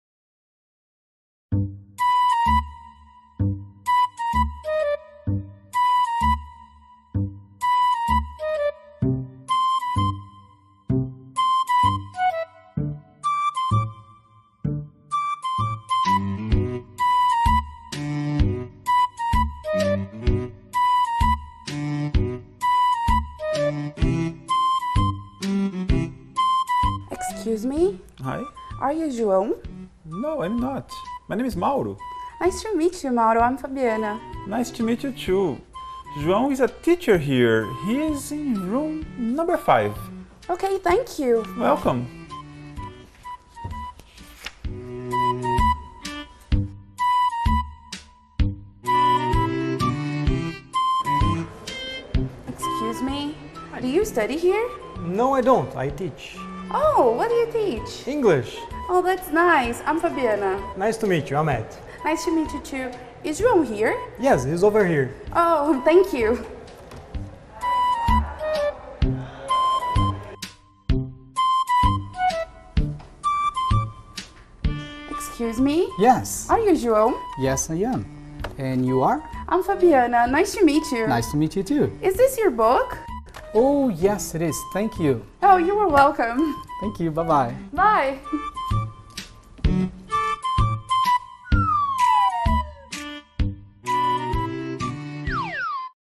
Unit 01 - Is this your book?
Aula criada para   o Centro de Mídias da Educação de São Paulo.

Transcript:
00:25-00:42
- Excuse me?
- Hi!
- Are you João?
- No, I am not. My name is Mauro.
- Nice to meet you, Mauro. I am Fabiana.
- Nice to meet you, too. João  is a teacher here. He is in room number 5.
- Ok, thank you.
- You’re welcome.

- Excuse me, do you study here?
- No, I don't,  I teach.
- Oh, what do you teach?
- English.
- Oh,  that's nice. I'm Fabiana.
- Nice to meet you. I’m Ed.
- Nice to meet you, too. Is João here?
- Yes, he is over here.
- Ok, Thank you.

- Excuse me?
- Yes!
- Are you João?
- Yes, I am. And you are?
- I’m Fabiana. Nice to meet you.
- Nice to meet you, too.
- Is this your book?
- Oh, yes, it is. Thank you.
- Oh, You are welcome.
- Thank you. Bye, bye.
- Bye.